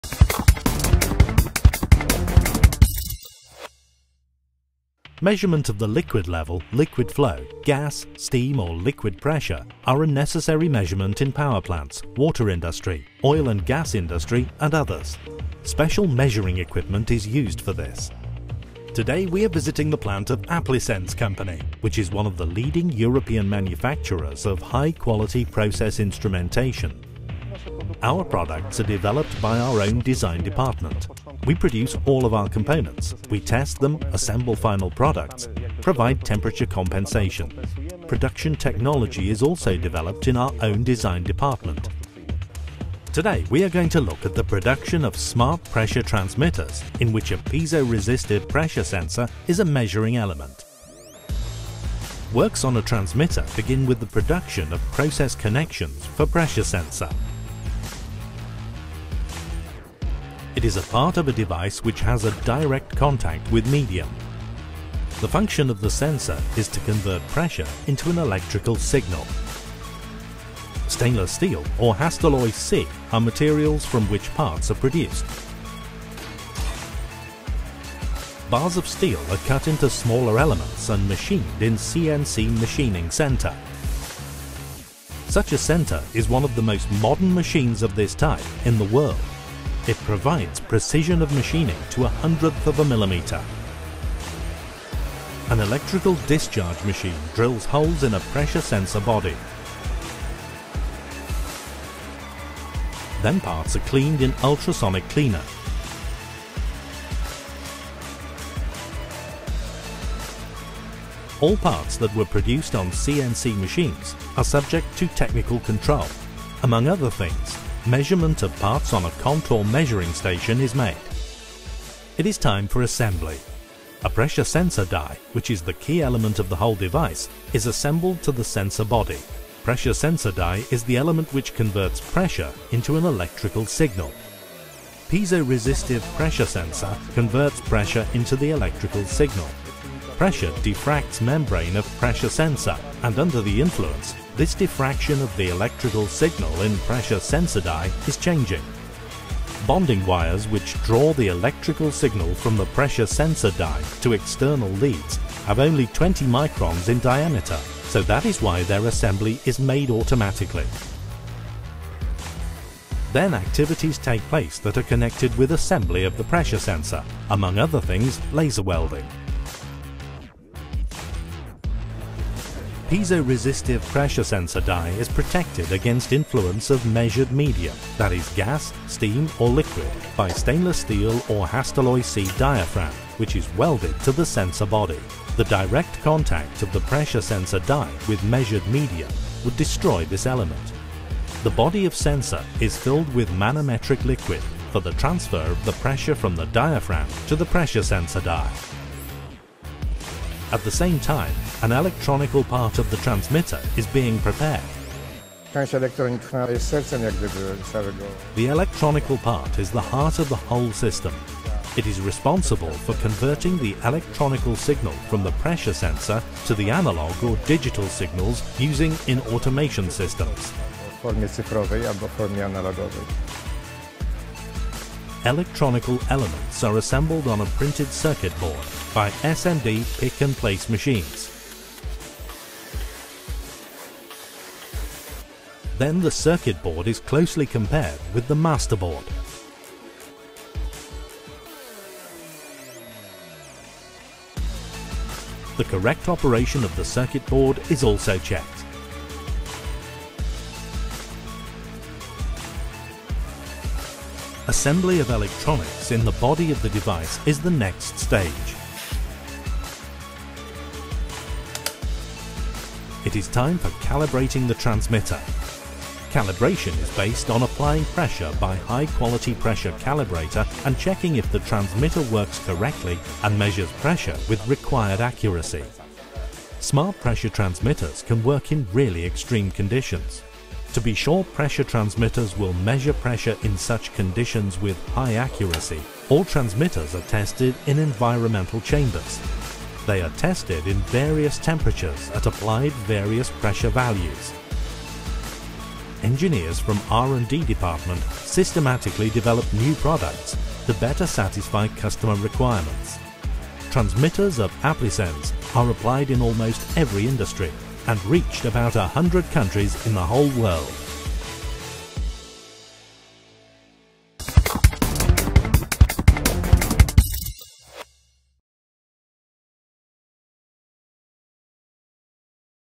Special Measuring Devices made by leading European manufacturer 🇪🇺
Measurement of the liquid level, liquid flow, gas, steam or liquid pressure are a necessary measurements in power plants, water industry, oil & gas industry and others. Special measuring equipment is used to this.

Today we are visiting the plant of Aplisens company which is one of the leading European manufacturers of high quality process instrumentation. Products are developed by our own design department. We produce all of our components, we test them, assemble final products, provide temperature compensation. Production technology is also developed in our own design department.

Today we are going to look at the production of smart pressure transmitters in which a piezoresistive pressure sensor is a measuring element. The electronical part is the heart of the whole system. It is responsible for converting the electronical signal from the pressure sensor to the analogue or digital signals using in automation systems.

00:00 Special measuring equipment
00:28 How measuring equipment is made
01:51 Inside factories
02:02 It is time for assembly.
02:52 Piezoresistive pressure sensor
04:23 Transmitter calibration